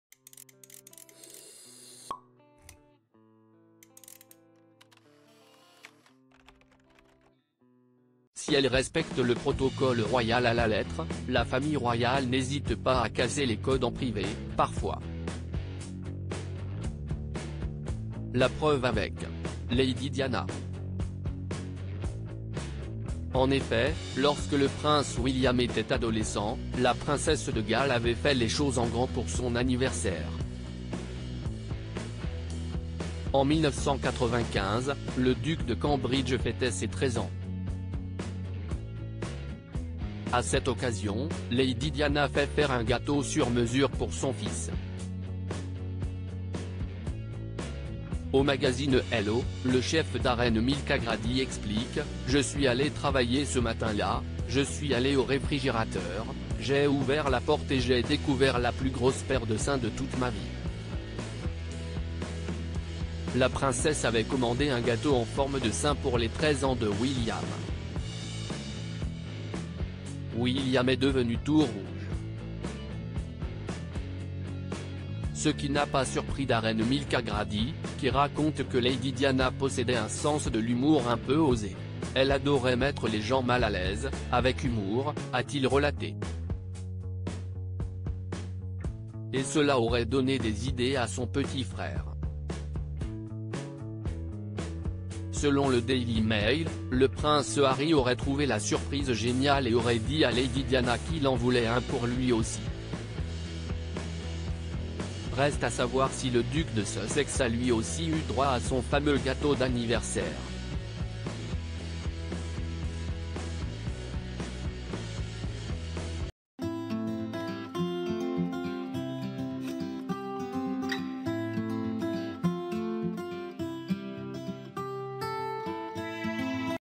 Quand Lady Diana a fait rougir le prince William avec une surprise très coquine pour son 13e anniver
Quand Lady Diana a fait rougir le prince William avec une surprise très coquine pour son 13e anniversaire